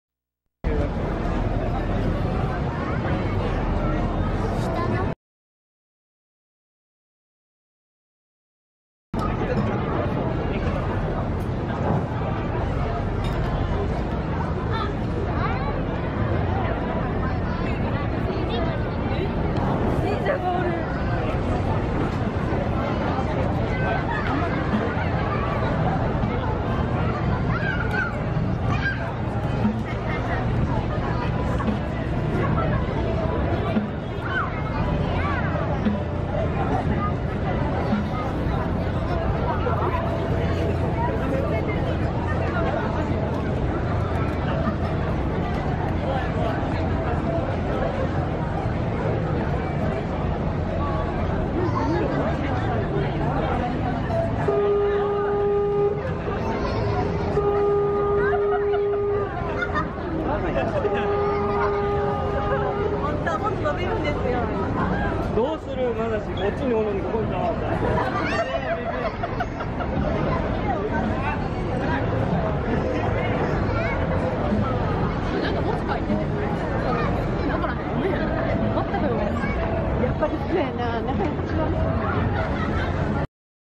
2017年5月5日こどもの日ライトアップの神戸ポートタワー 鯉のぼりと吹き流しカラーのライトアップの
鯉のぼりの吹き流しの色にライトアップされた
神戸ポートタワーです
汽笛も聞こえて、港の風景ですね